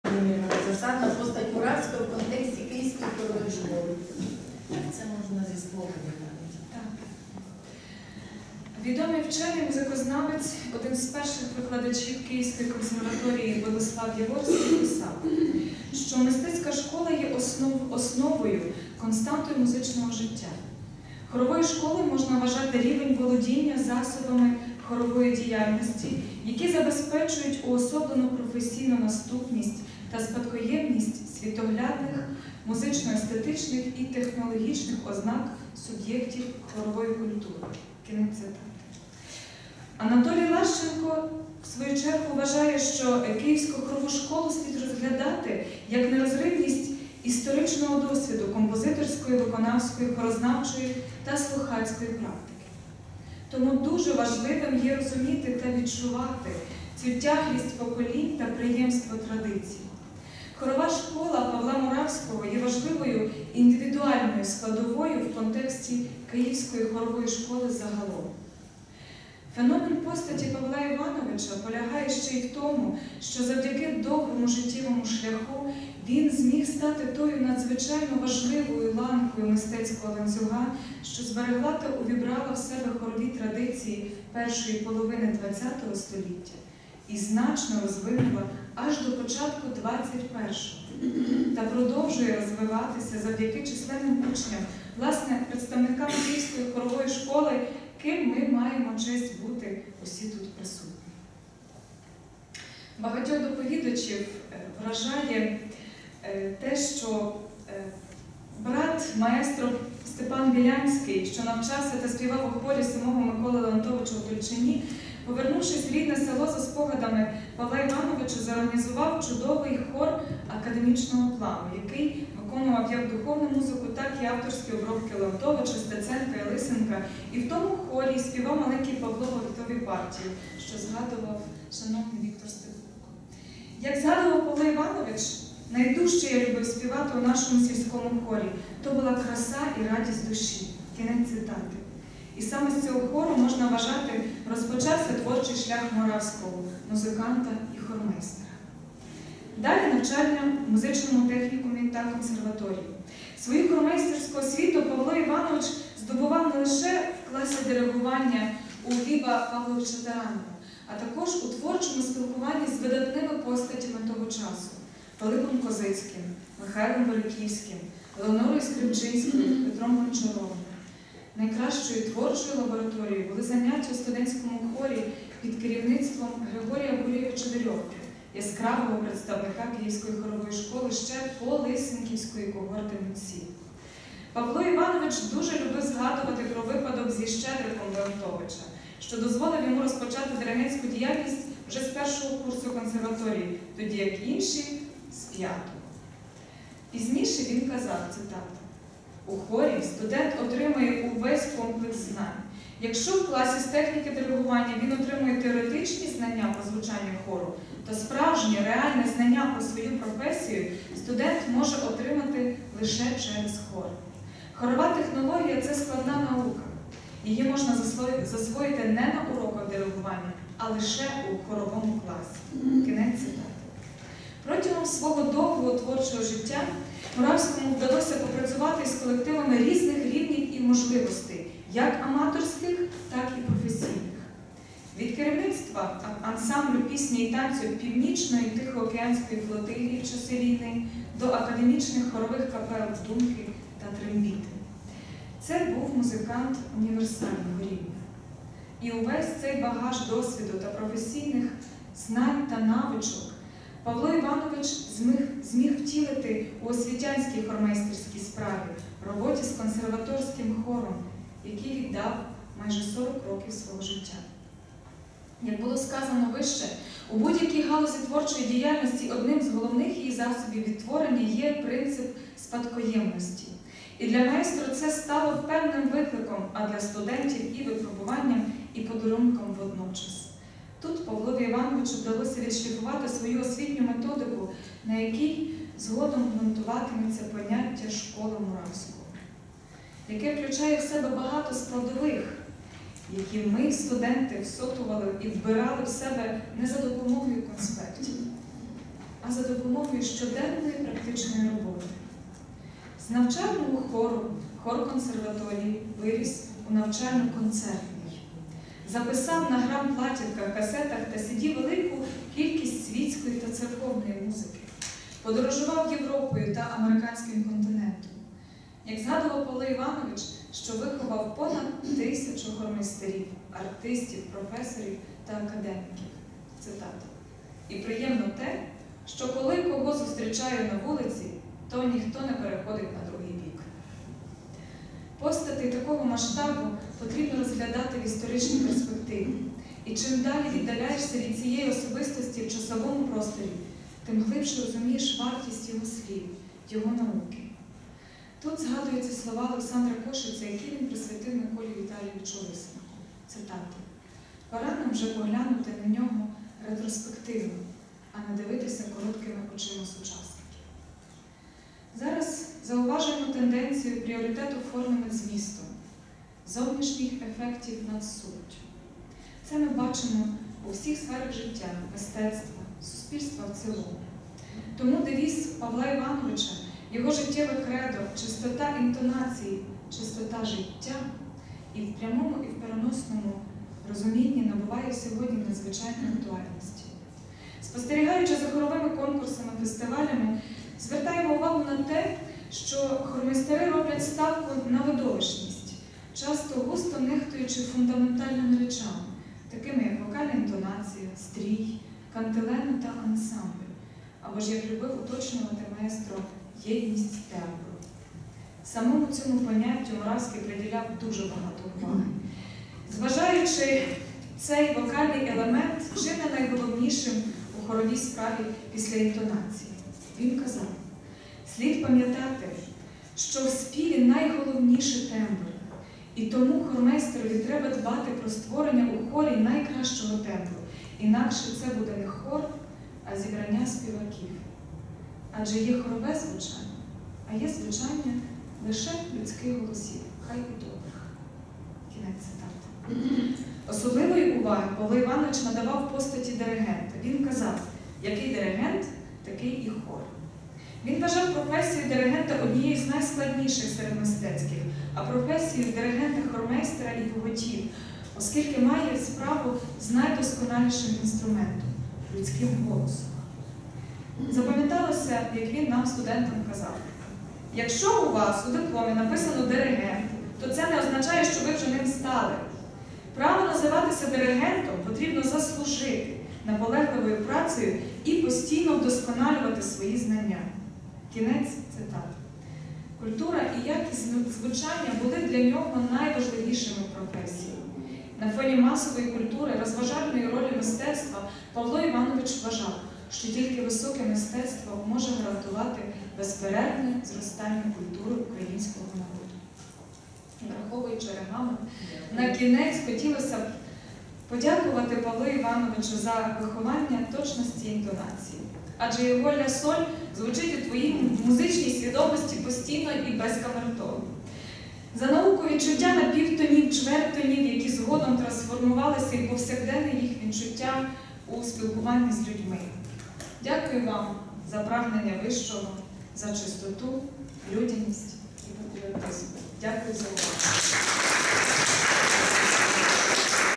Ольга Засадна: Постать Павла Муравського у контексті діяльності Київської хорової школи.
Засадна Ольга Володимирівна, кандидат мистецтвознавства, заслужена
артистка України, старший викладач кафедри хорового дириґування
Національної музичної академії України імені П. І. Чайковського
Постать Павла Муравського у контексті діяльності Київської хорової школи.

Всеукраїнська науково-практична конференція «Хорова школа Павла Муравського в контексті української музичної культури», до 105-ліття заснування Київської консерваторії (НМАУ імені П. І. Чайковського) та 105-ліття народженняй 5-х роковин пам’яті Павла Івановича Муравського